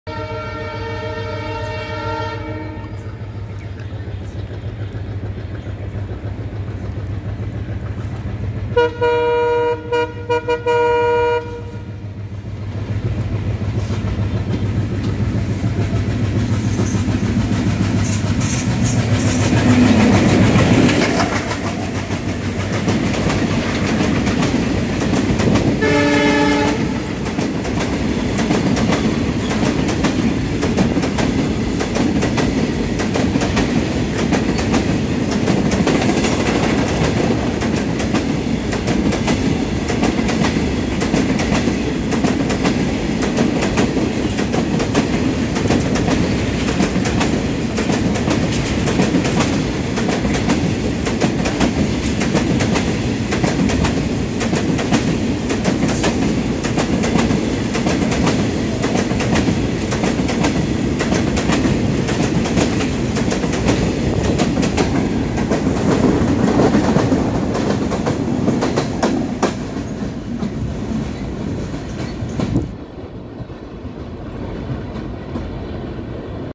WDM3D locomotive pulling heavy loaded wagons | ভারতের উপহার দেওয়া লোকোমোটিভের দুর্দান্ত গতি
Empty BCNAHSM1 returning to India at high speed.
Locomotive:
The Indian locomotive class WDM-3D is a class of diesel-electric locomotive that was developed in 2003 by Banaras Locomotive Works (BLW), Varanasi for Indian Railways. The model name stands for broad gauge (W), Diesel (D), Mixed traffic (M) engine with 3300 horsepower (3D). The engine is classified WDM-3D though it outputs only 3300 hp and not 3400 hp as the name should suggest. They entered service in 2003. A total of 590+ WDM-3D were built at Banaras Locomotive Works (BLW), Varanasi between 2003 and 2016.
Wagon :
Description Specification
Track Gauge 1676.00 mm
Total Weight (Gross Load) 91.6 M.T.
Tare Weight 24.6 M.T.
Load Capacity 67.0 M.T.
Volume Capacity 103.4 cu.m.
Length Over Head Stock 13521.00 mm
Length Over Coupling Faces 14450.00 mm
Overall Width (Over loaded) 3200.00 mm
Overall Height from Under Tare 4017.00 mm
Coupler Type High Tensile Centre Buffer Coupler (N.T.)
Bogie Type CASNUB 22 HS BOGIE - 92
Brake System Bogie Mounted Brake System
To Transport Cement/ Crops & Cereals